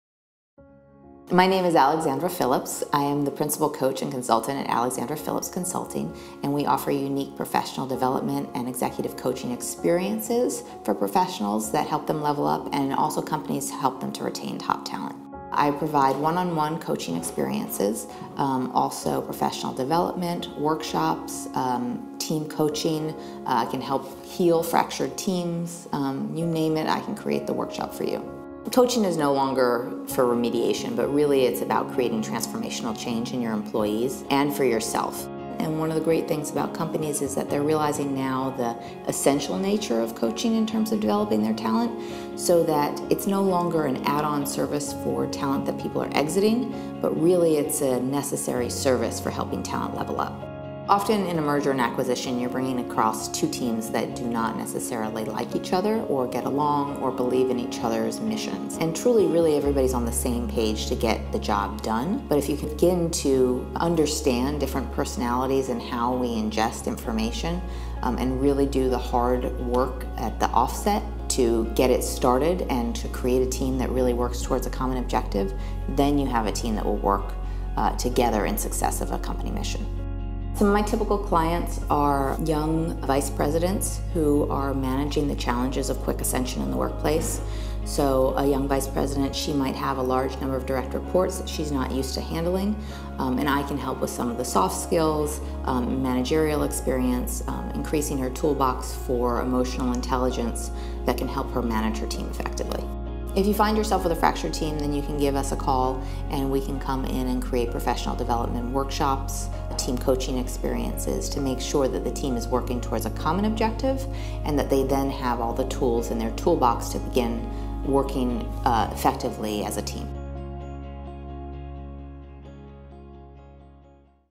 Meet Alexandra Phillips | Forbes Coaches Council
Alexandra Phillips has over fifteen years of experience in business performance coaching, consulting, and public speaking. Alexandra specializes in helping leaders to help develop their leadership, management, and negotiation skills.  She has worked with executives through large scale M&A, as well and large and small re-orgs, helping to find solutions to heal fractured teams.